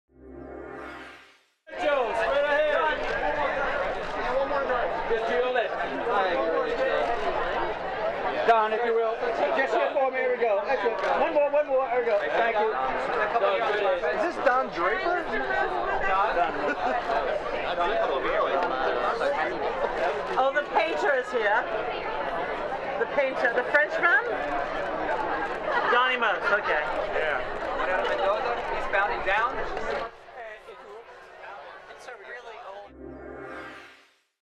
Don Most - Hub Network's First Annual Halloween Bash - Purple Carpet Arrivals
Don Most HUB Network's First Annual Halloween Bash Purple Carpet Arrivals at Barker Hangar in Santa Monica, Ca USA October 20, 2013